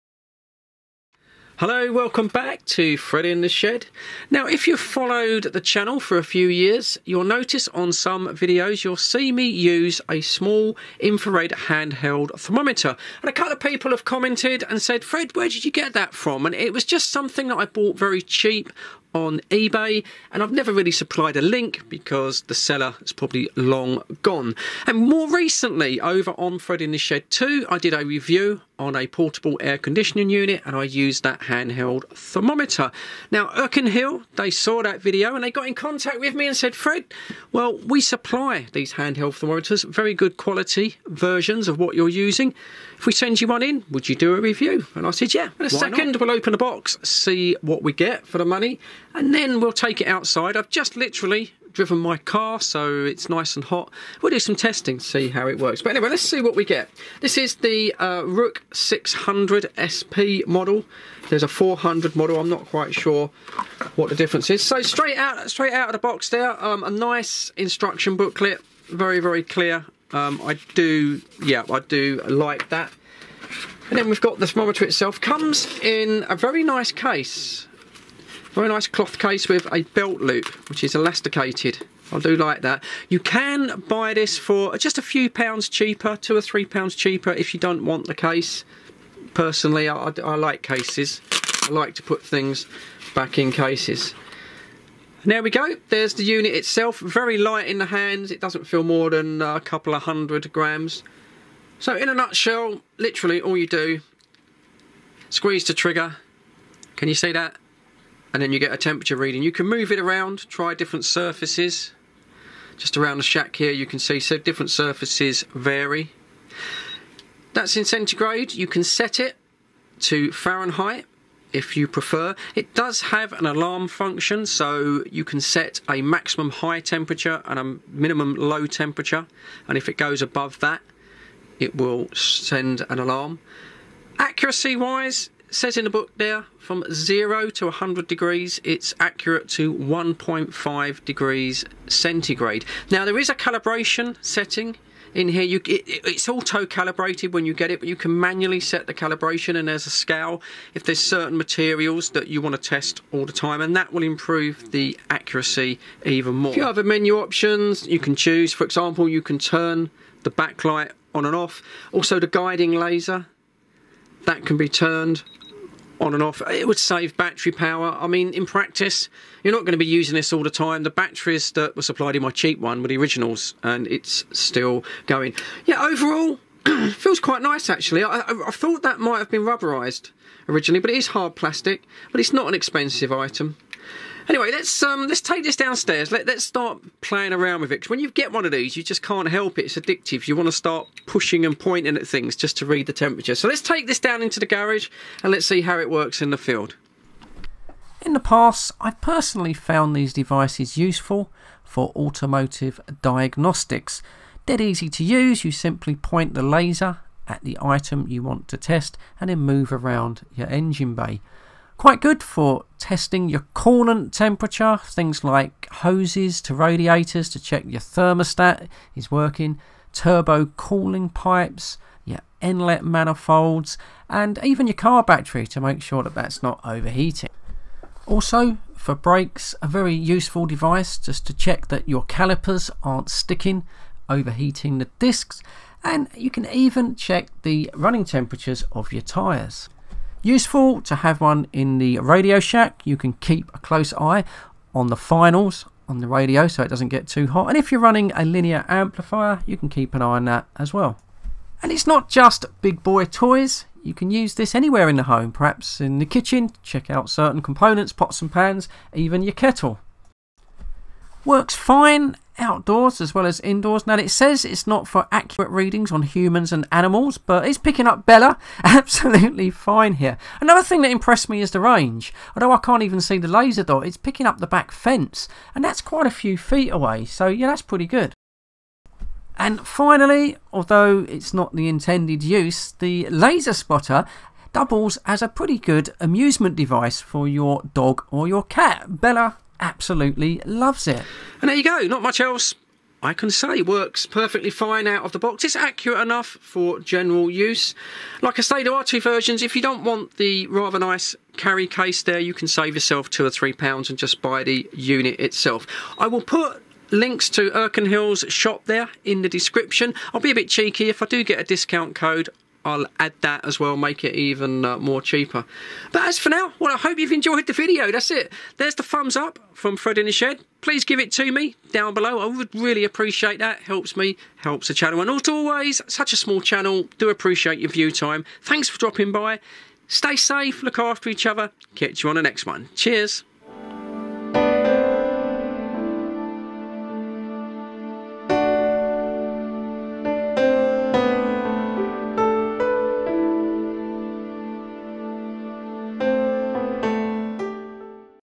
ERICKHILL Digital Infrared Thermometer Gun. Full review.
ERICKHILL Digital Infrared Thermometer -50℃~600℃ Non-Contact Laser Temperature Gun LCD Backlit Display Adjustable Emissivity. Review and testing.
15% discount code. Code No: DBZWISAZ  End Date: 21/10/2022 23:59

【High Accuracy】: Combining the latest temperature sensor technology and advanced Fresnel lens, this instrument reads values of high accuracy ±1.5°/±1.5% in only 0.5s response; The adjustable emissivity is 0.1-1.0, which can measure the surface temperature of different objects more precisely.
【Precisely Locating】: Built-in red laser for precisely aiming, accurately finding the measuring target; Blue screen with black digital display, clear reading. The distance to spot ratio is 12:1, which means you can measure the target at a longer distance.
【Multi Functions】: This infrared thermometer could test the temperature during --50℃~600℃/(-58℉~1112℉); Switch between ℃/℉ unit as needed; with LCD backlight allows you can check the temperatures even in the dark condition; Auto power off after 12’S no operation which helps save battery power.

Colour 600℃ Black Blue
Material ABS
Measurement System Laser Sensor
Model Number ROOK 600SP-W
Part Number Rook 600SP
Power Source Type Battery Powered